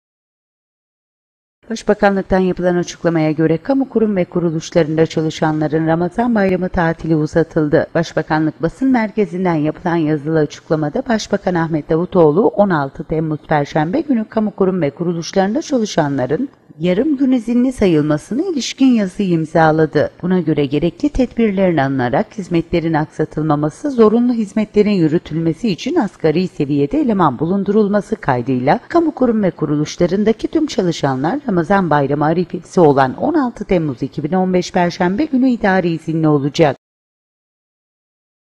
RAMAZAN BAYRAMI KAÇ GÜN?
Başbakanlıktan yapılan açıklamaya göre, kamu kurum ve kuruluşlarında çalışanların Ramazan Bayramı tatili uzatıldı.